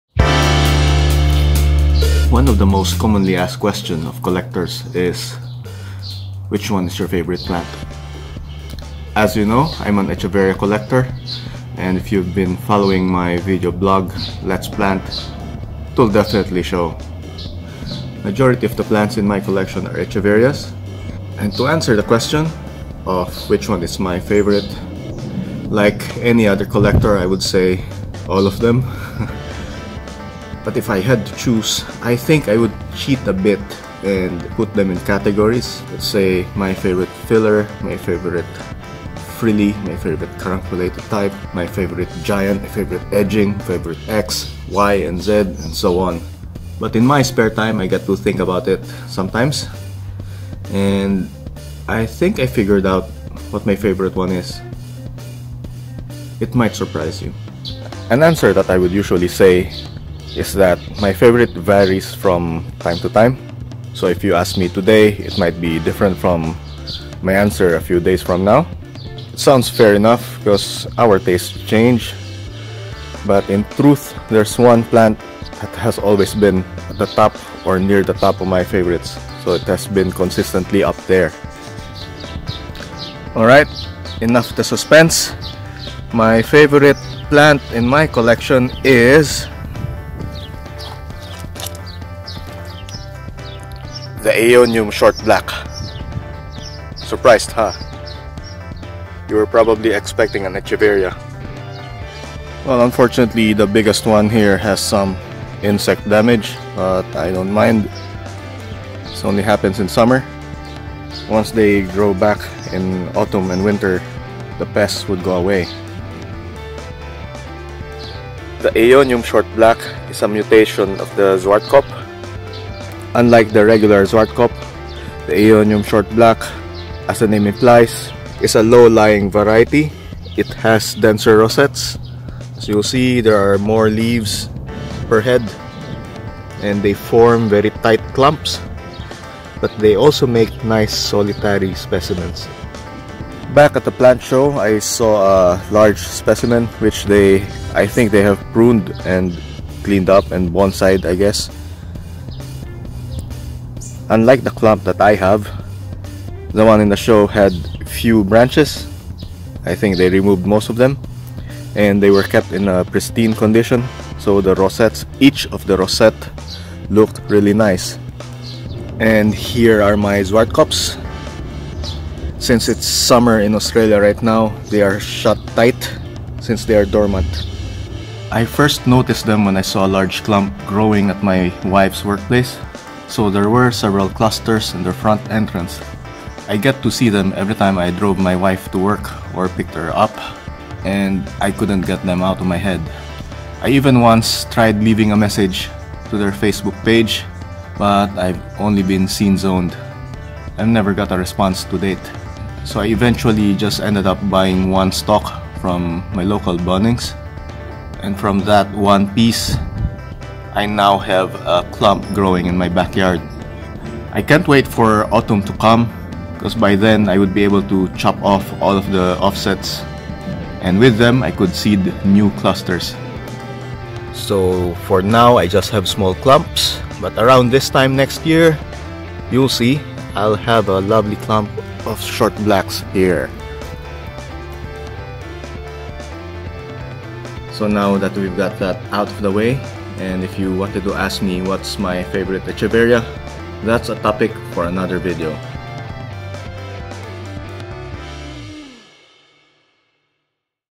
[LP Bites] What is my favourite plant?
LP Bites
What is my favourite plant?

This is a commonly asked question asked of collectors, and most will say that they have a hard time picking.

Luckily I had a chance to think about it a while back... so here it is!